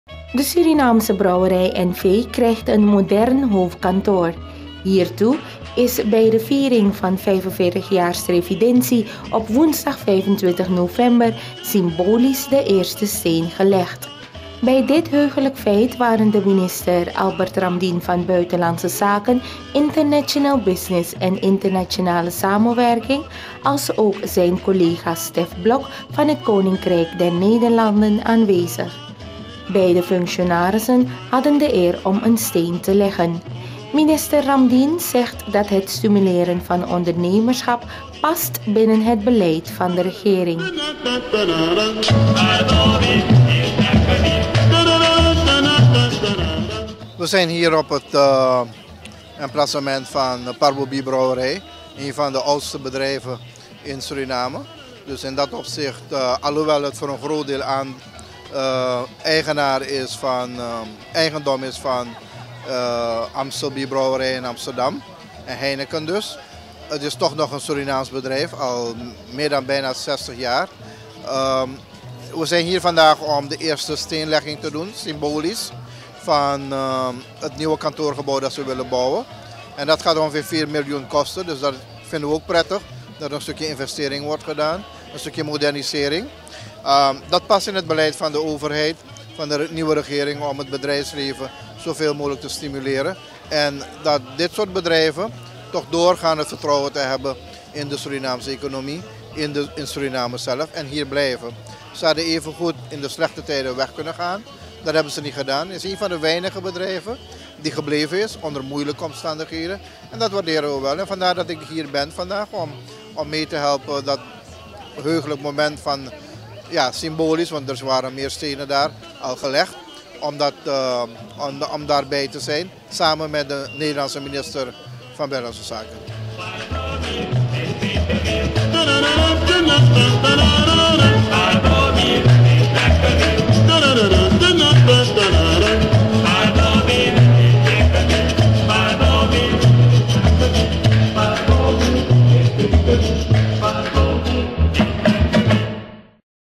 SYMBOLISCHE STEENLEGGING NIEUW HOOFDKANTOORGEBOUW BIERBROUWERIJ
De Surinaamse Brouwerij NV krijgt een modern hoofdkantoor. Hiertoe is bij de viering van 45 jaar Srefidensi op woensdag 25 november symbolisch de eerste steen gelegd. Bij dit heugelijke feit waren de ministers Albert Ramdin van Buitenlandse Zaken, International Business en Internationale Samenwerking alsook zijn collega Stef Blok van het Koninkrijk der Nederlanden aanwezig. Beide functionarissen hadden de eer een steen te leggen. Minister Ramdin zegt dat het stimuleren van ondernemerschap pas binnen het beleid van de regering.